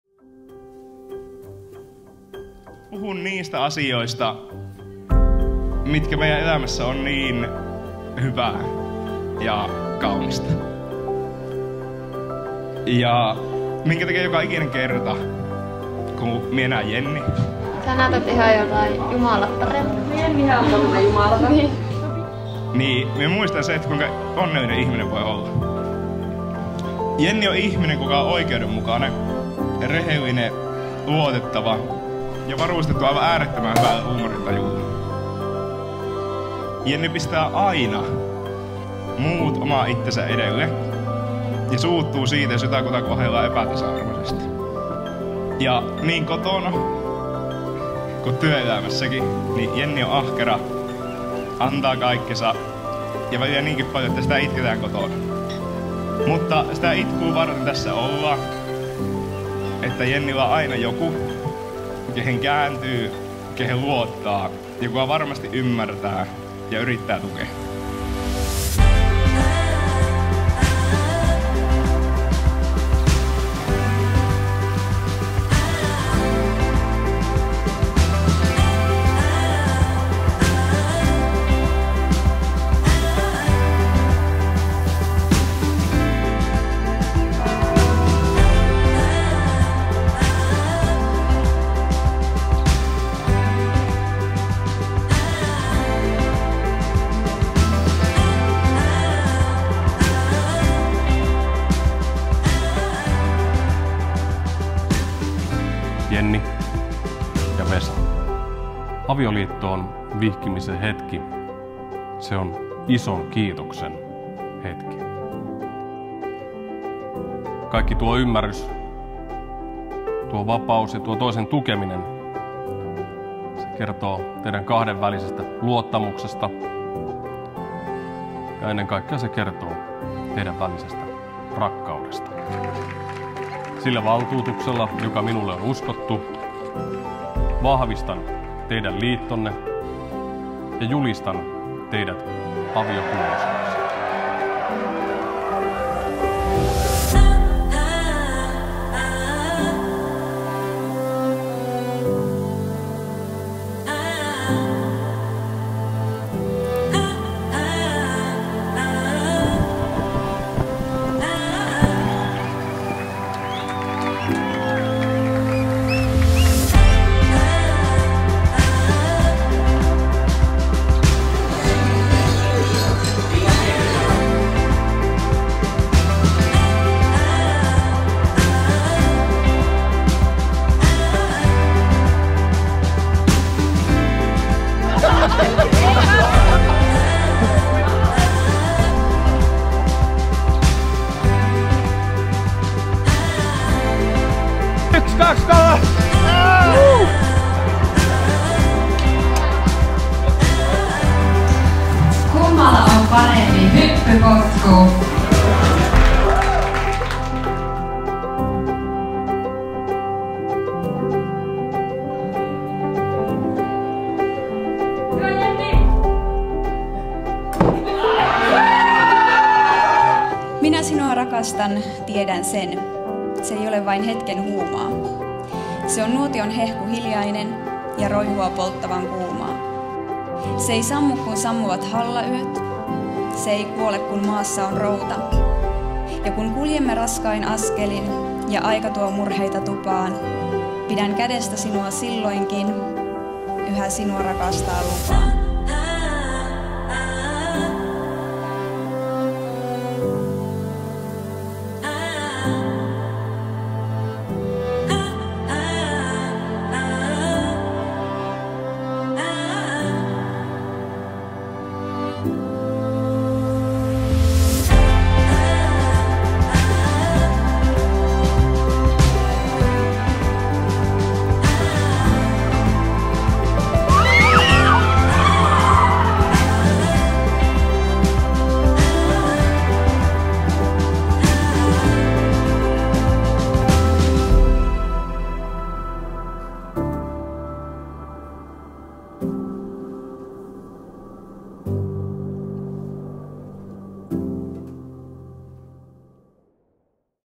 Jenni&Vesa 17.7.2021 häävideo
Hääpaikkana Hannuksen piilopirtti Tyrnävällä